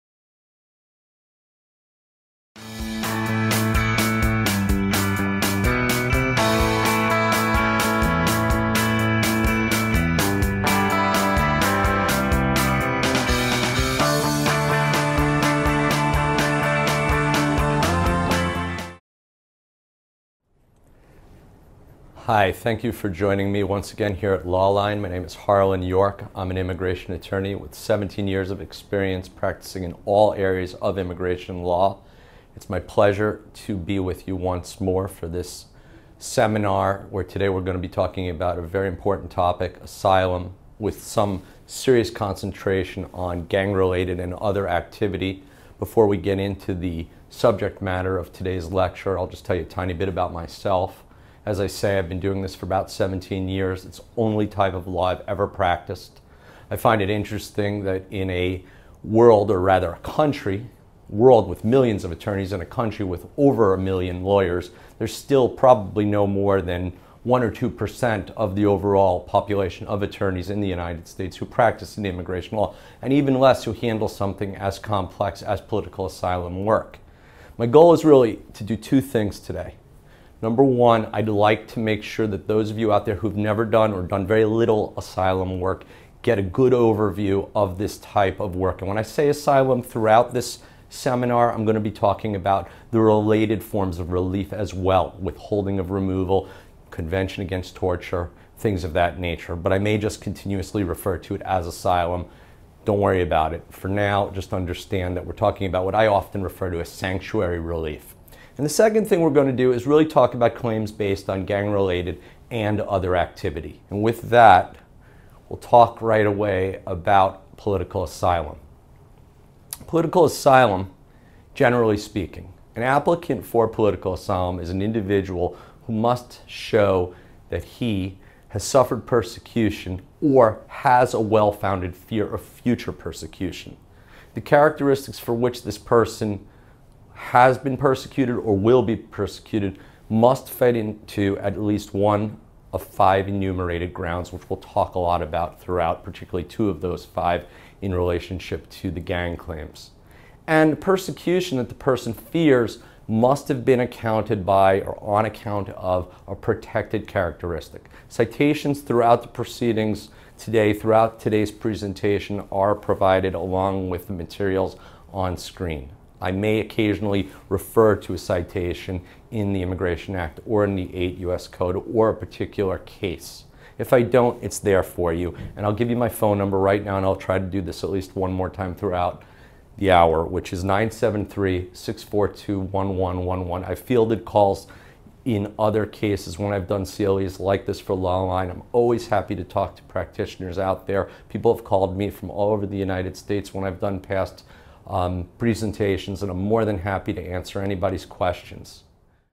"Political Asylum and Gang Activity," Harlan York
Course Description:
Individuals who seek asylum on the basis of gang related violence have the option of claiming membership in a particular social group or on the basis of political opinion. Join us as Lawline Faculty Member, Harlan York, takes us through the relationship between asylum law and gang activities. Mr. York will begin his presentation by discussing the basic of asylum law and current case law. Finally, Mr. York will conclude his presentation by discussing statutory regulations, withholding, the convention against torture, and current cases.